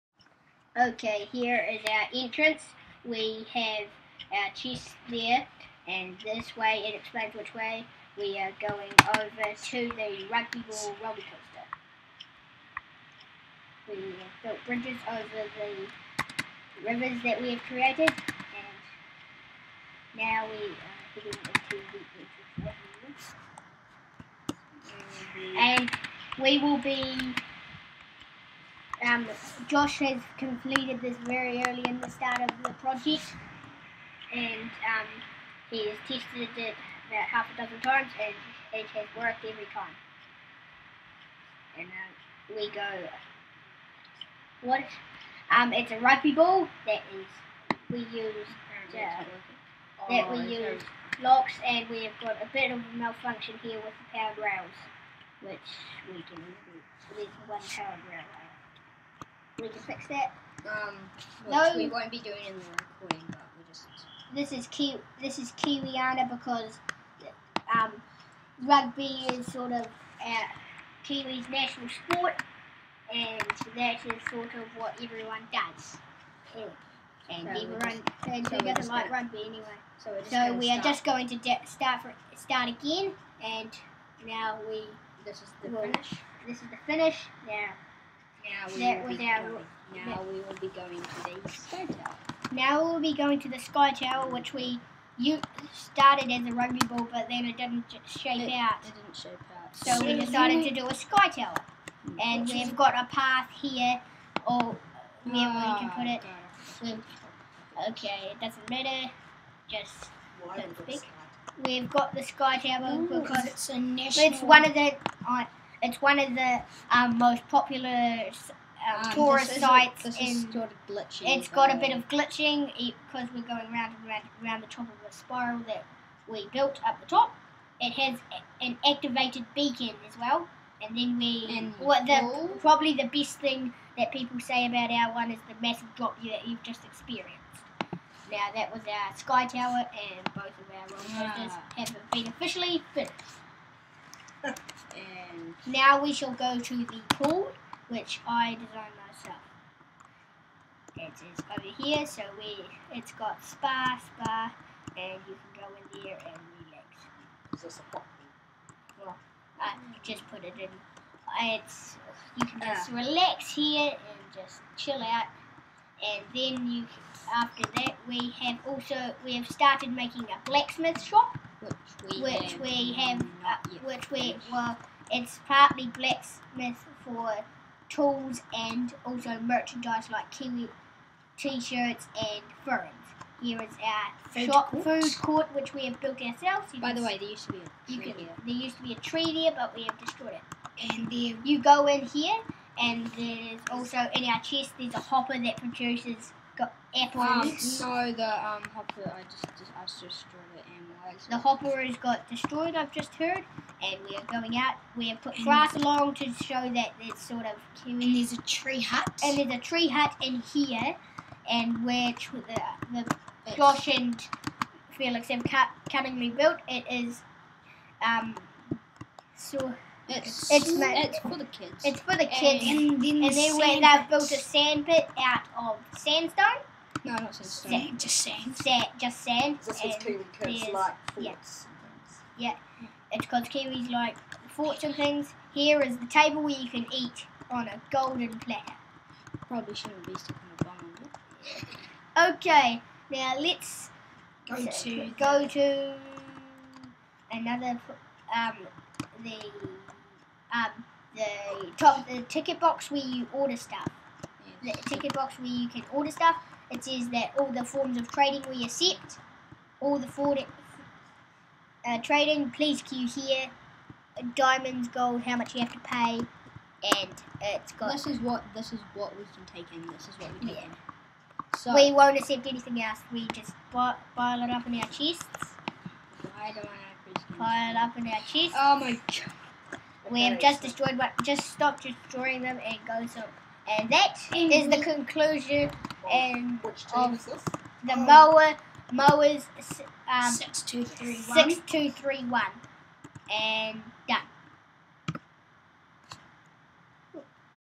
Moas631 Final Tour
The Moas631 Team's final tour of their section of the Kiwiana Theme Park.